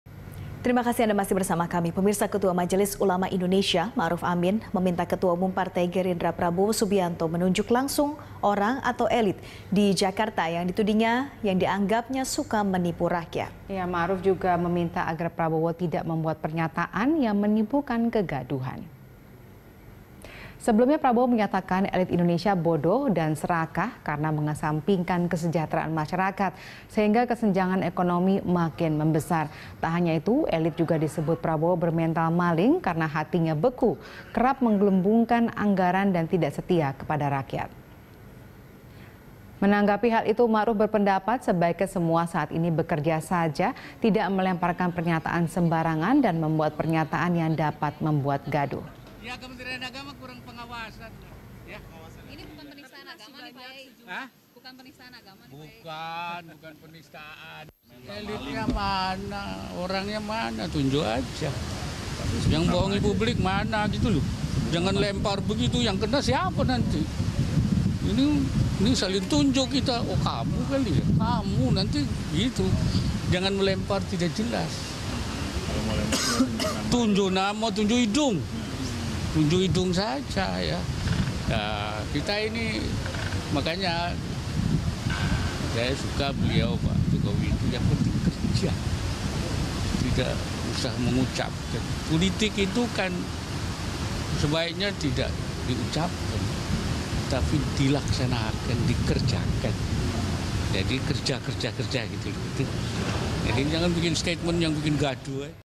Ketua MUI Minta Prabowo Sebut Nama Elite Goblok dan Maling
Ketua Majelis Ulama Indonesia Ma’ruf Amin meminta Ketum Partai Gerindra Prabowo Subianto menunjuk langsung orang atau elite di Jakarta yang ditudingnya yang dianggapnya suka menipu rakyat. Ma’ruf juga meminta agar Prabowo tidak membuat pernyataan yang menimbulkan kegaduhan.

Facebook.com/BeritaSatuTV
Youtube.com/BeritaSatu